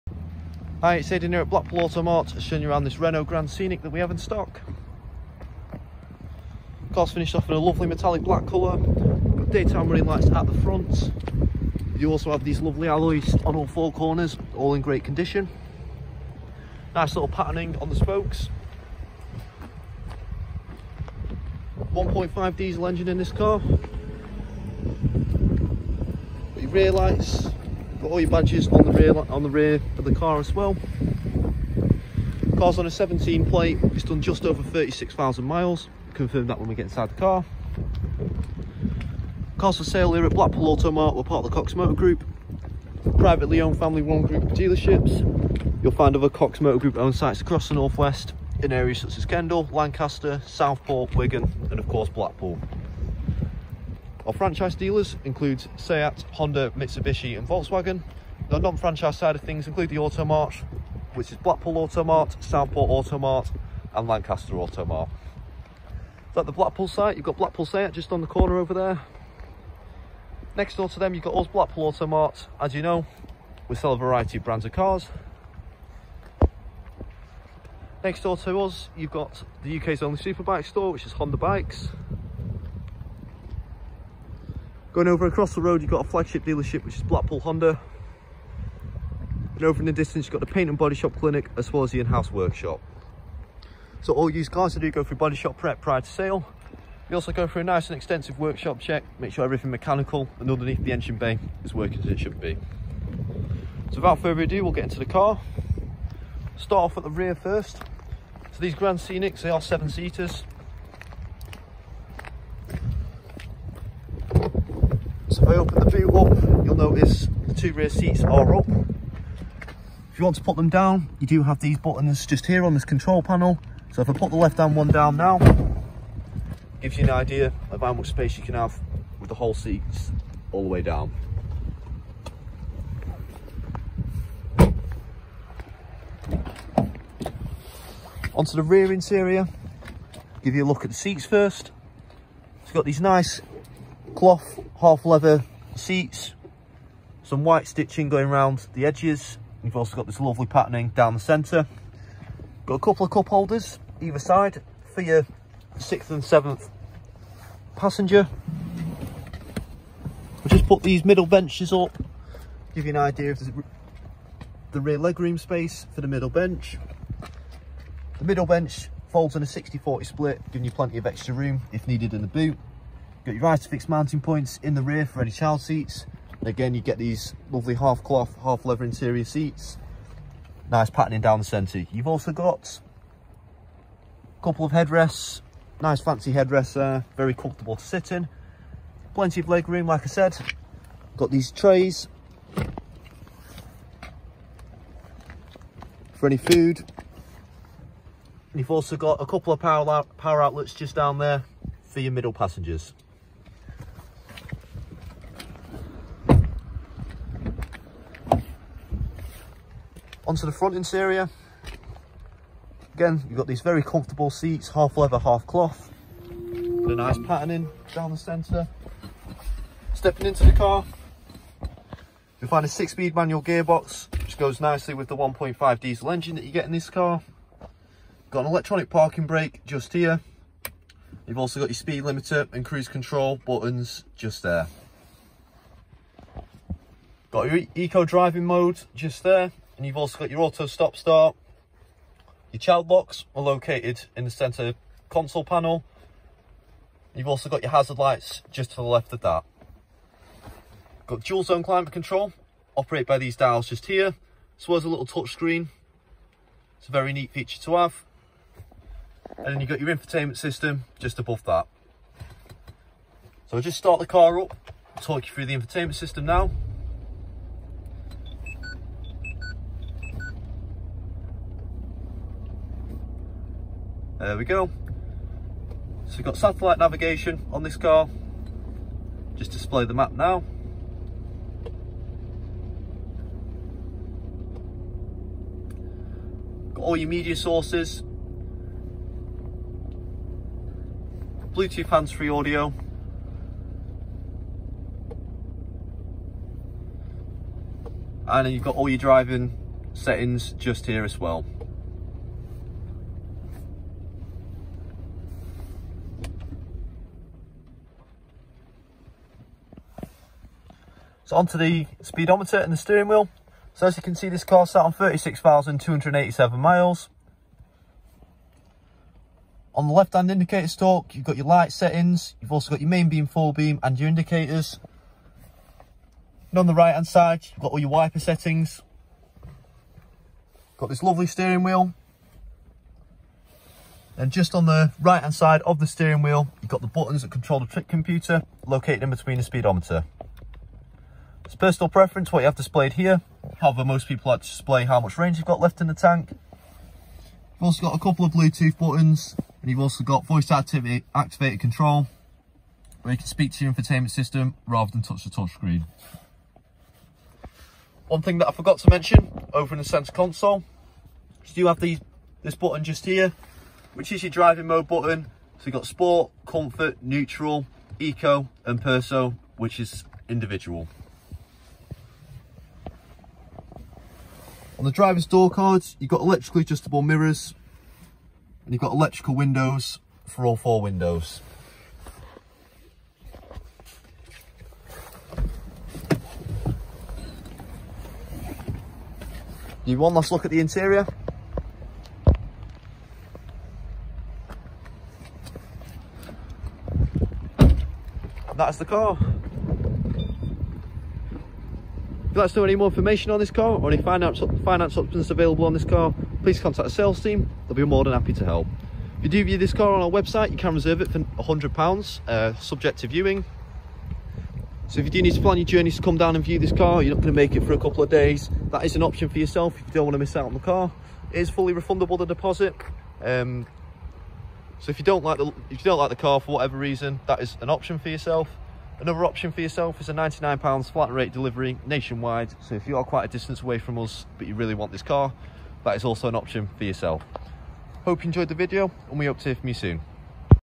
Renault Grand Scenic 1.5 dCi Dynamique Nav |Blackpool Automart
Renault Grand Scenic 1.5 dCi Dynamique Nav.

20in Alloy Wheels - Silverstone
AEBS - Advanced Emergency Braking System with Pedestrian Detection
Airbags - Curtain Front and Rear
Airbags - Front Lateral
Airbags - Front Passenger
Android Auto
Apple CarPlay
Arkamys Auditorium 3D Sound System with 6 Speakers
Automatic Headlights and Windscreen Wipers
Cruise Control with Speed Limiter
Display - Digital Speedometer - 7in TFT Screen
ESP - Electronic Stability Programme with Understeer Control
Electronic Parking Brake
Front Fog Lights
Headlights - Halogen
Insurance Approved - Category 1 Alarm System and Immobiliser
Parking Sensors - Front and Rear
R-Link 2 Multimedia System - 7in Touchscreen
Roof Rails - Longitudinal
Start and Stop Button
Tinted Windows